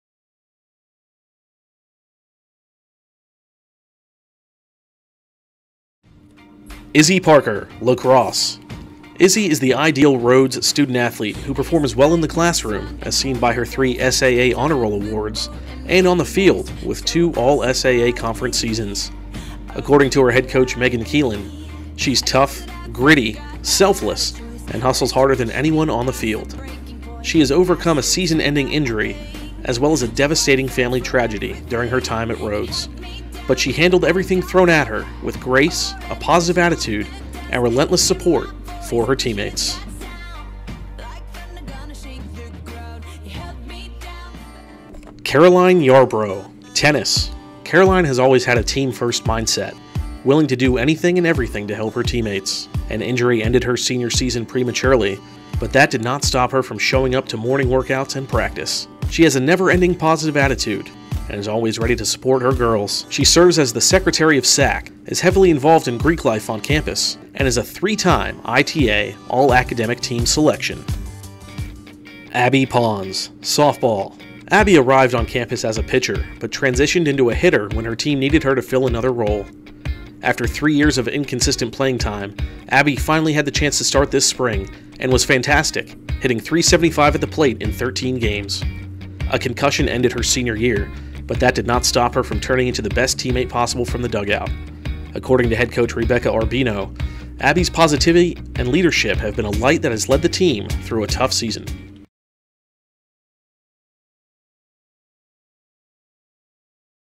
2022 Lynx Award: George Lapides Award (Female)
Congratulations to our female George Lapides Sportsmanship award finalists!
The George Lapides Sportsmanship Award is given each year to a senior male and a senior female student-athlete who best exemplifies the highest level of sportsmanship to his/her team.

Winner: Izzy Parker